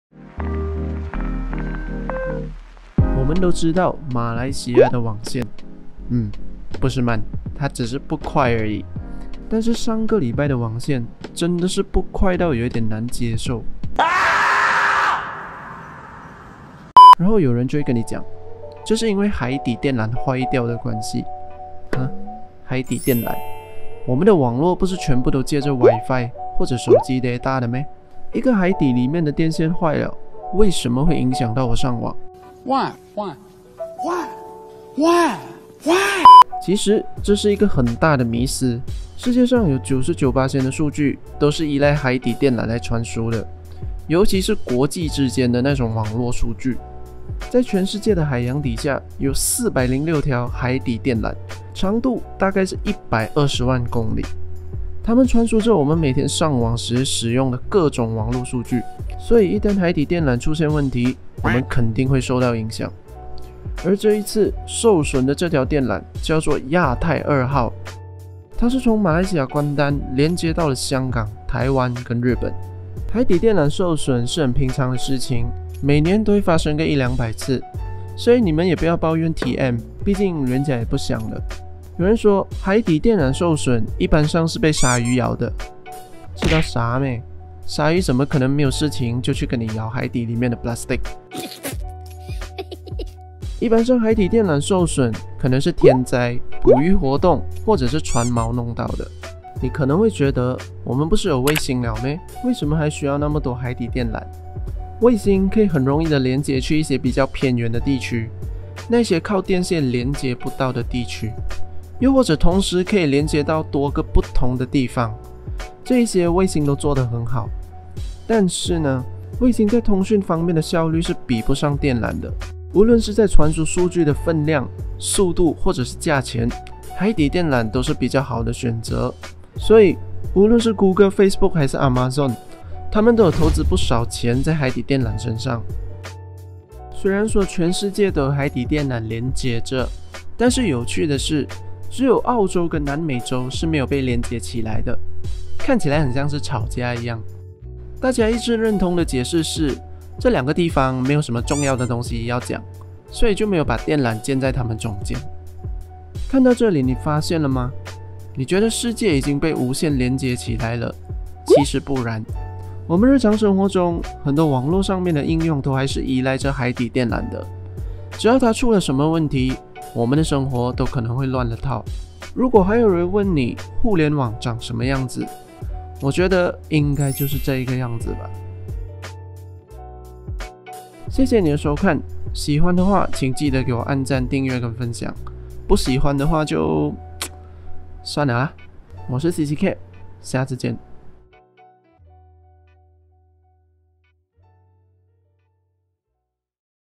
海底电缆断了你就上不到网 | 互联网的真面目 | Wireless World Is A Myth, We Still Need Submarine Cables
带你看看互联网长什么样子